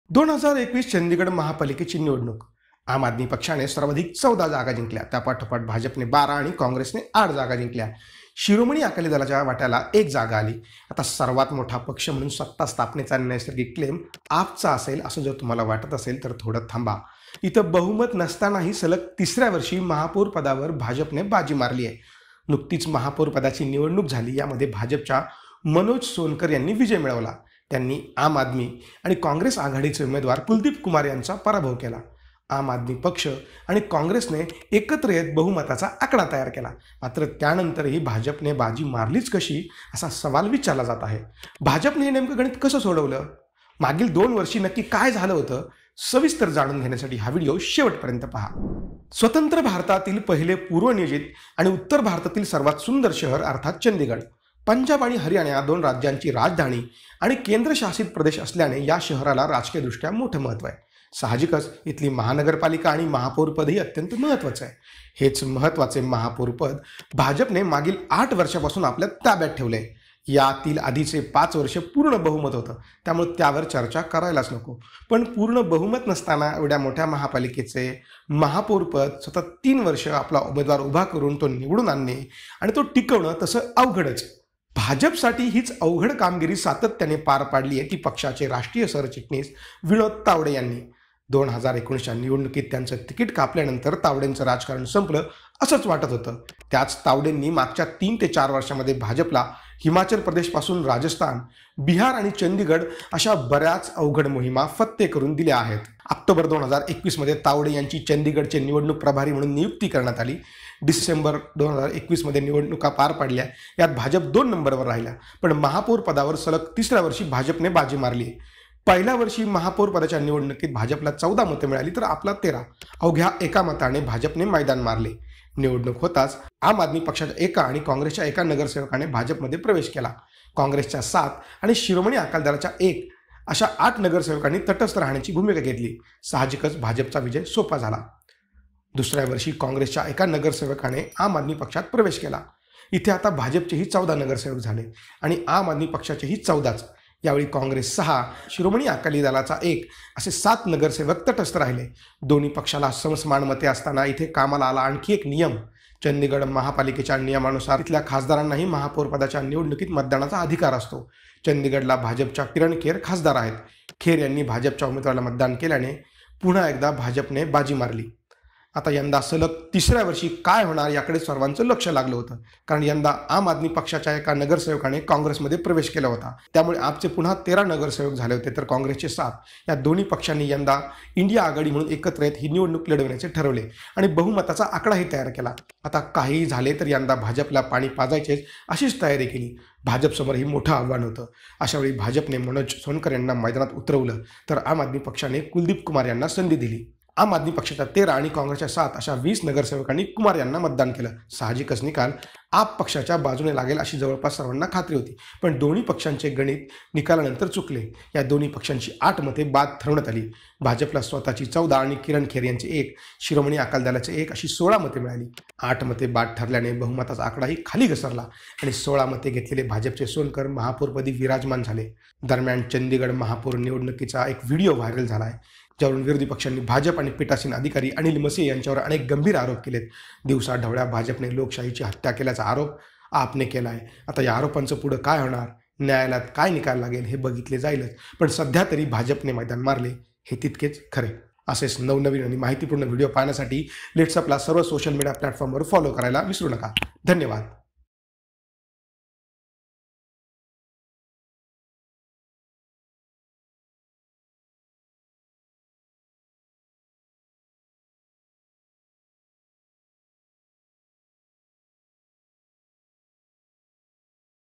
Chandigarh Election : चंदीगडमध्ये बहुमत नसताना भाजपने मोहीम कशी फत्ते केली? | LetsUpp Marathi
चंदीगडच्या महापौर निवडणुकीत आम आदमी पक्ष आणि काँग्रेसने एकत्र येत बहुमताचा आकडा तयार केला होता. मात्र त्यानंतरही भाजपने बाजी कशी मारली? असा सवाल विचारला जात आहे. भाजपने नेमके हे गणित कसे सोडविले? मागील दोन्ही वर्षी काय झाले होते? सविस्तर जाणून घेण्यासाठी व्हिडीओ शेवटपर्यंत पाहा...